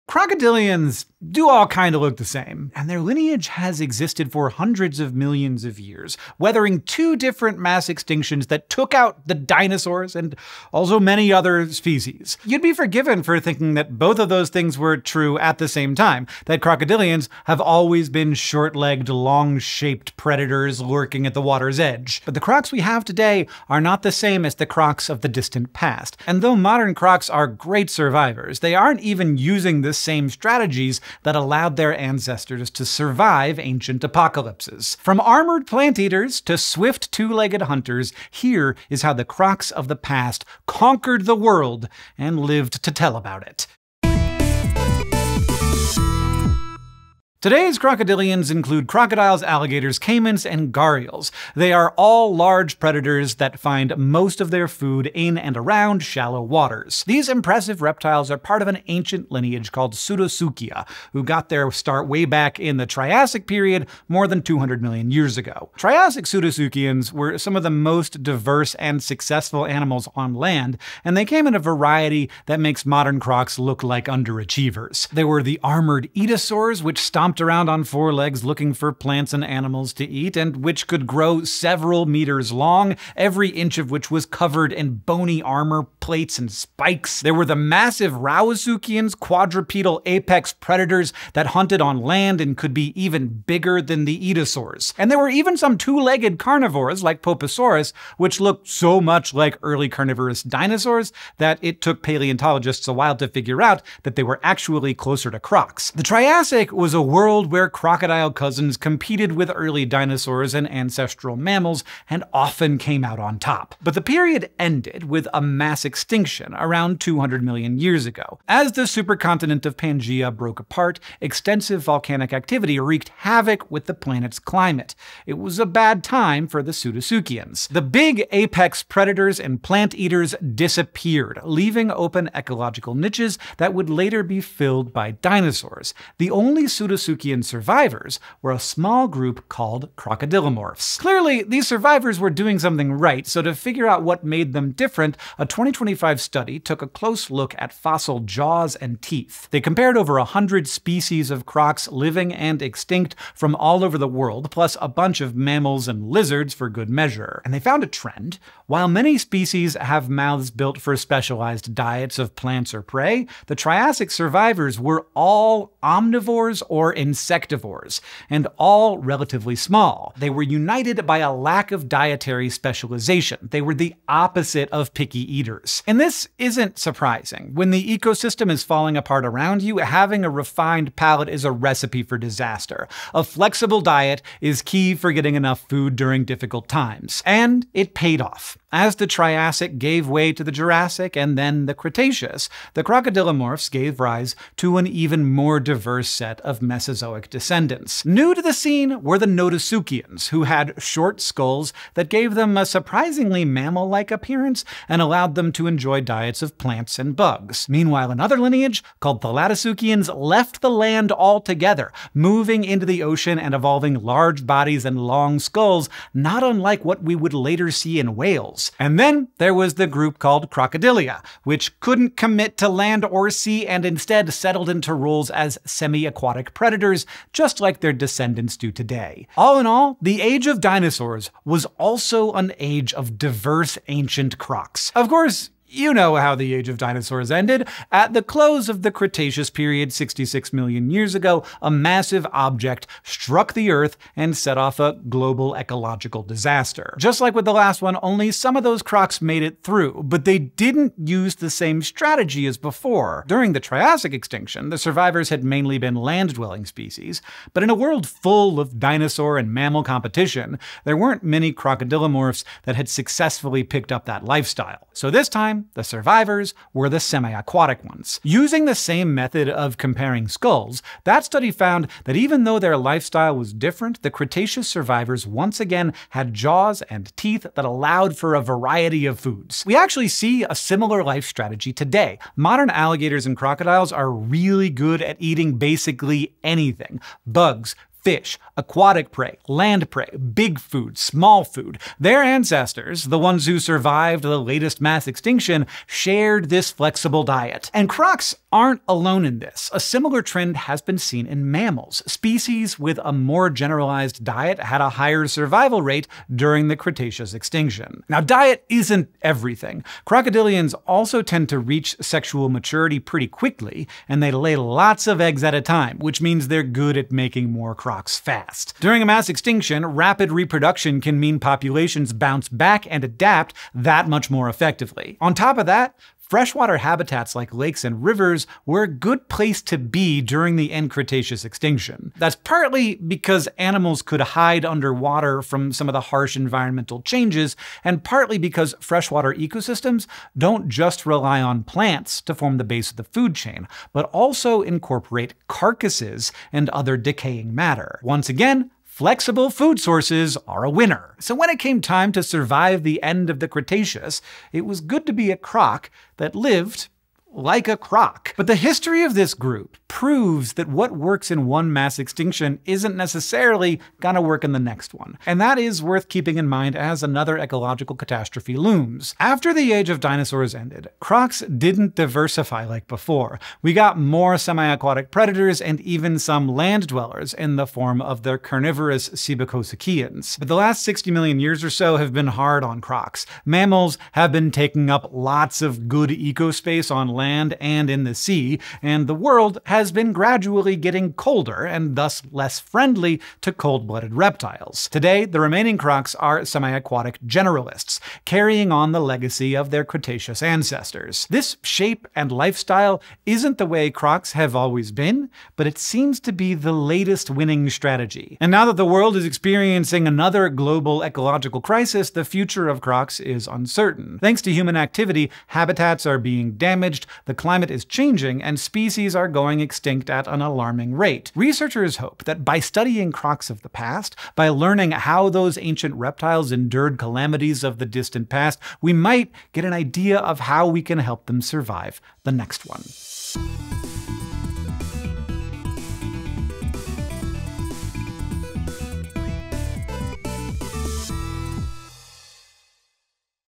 How Crocodilians Just Keep on Surviving
All crocodilians look more or less the same today, but to survive two different mass extinctions, they've had to change a lot. Here's how they pulled it off.